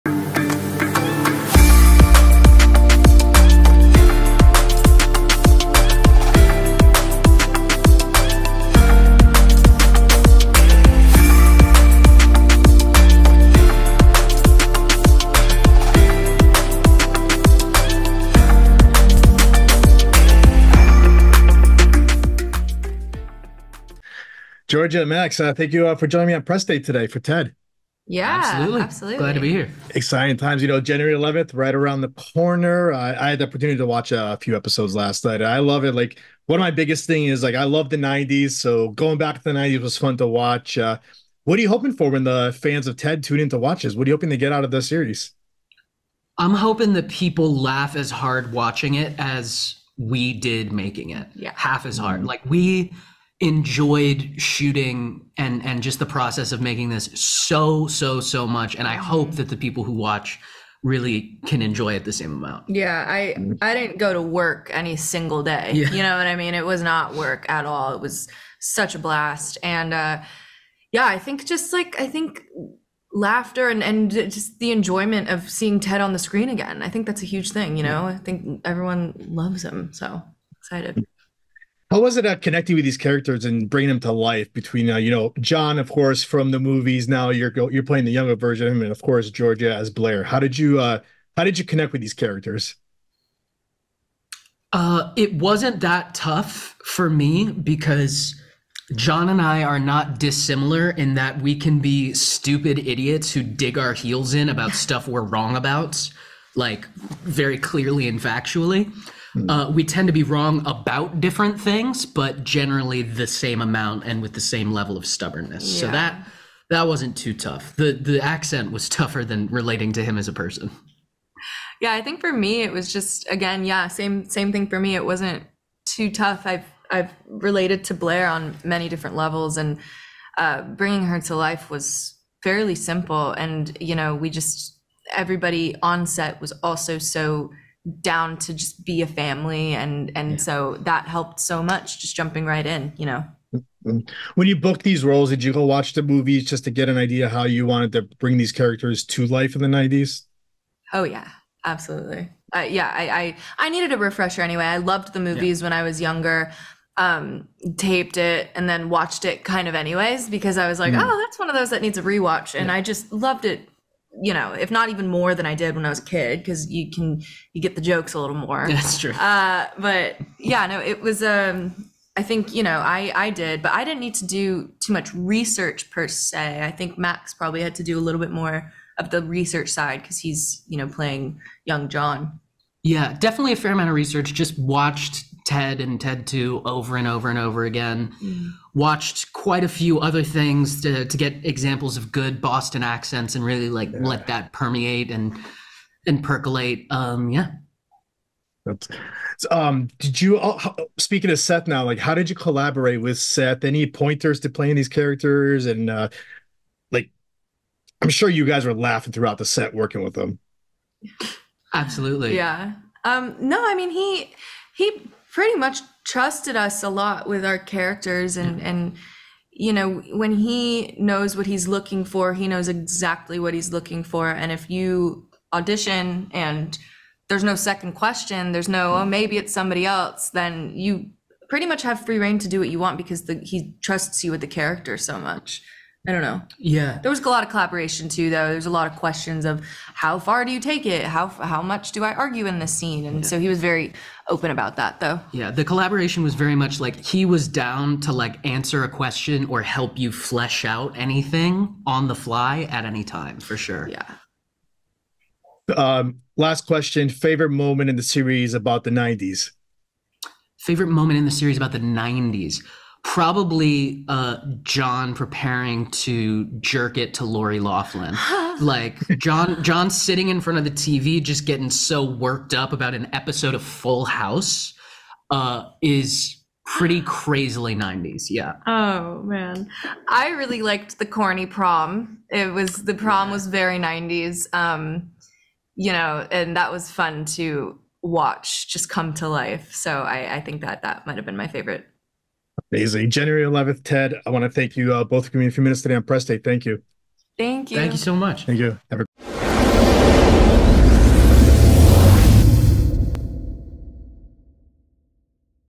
Max Burkholder and Giorgia Whigham Dish on 'Ted' Prequel
Join Max Burkholder, who portrays John Bennett (Mark Wahlberg's younger self), and Giorgia Whigham as Blaire, the talented young stars of Peacock's Ted prequel series, in an exclusive interview with Elias on press day. In this candid conversation, they share insights into the upcoming show, discuss their favorite moments, and provide a sneak peek into the world of the Ted prequel. Dive into the behind-the-scenes revelations and get ready for an exciting glimpse into the lives of these rising stars and the highly anticipated Ted series.

TED - In this prequel series to the Ted films, it's 1993, and Ted the bear’s (Seth MacFarlane) moment of fame has passed. He’s now living back home in Framingham, Massachusetts with his best friend, 16-year-old John Bennett (Max Burkholder), along with John’s parents, Matty and Susan (Scott Grimes and Alanna Ubach) and cousin Blaire (Giorgia Whigham). Ted may be a lousy influence on John, but at the end of the day, he’s a loyal pal who’s always willing to go out on a limb for friendship.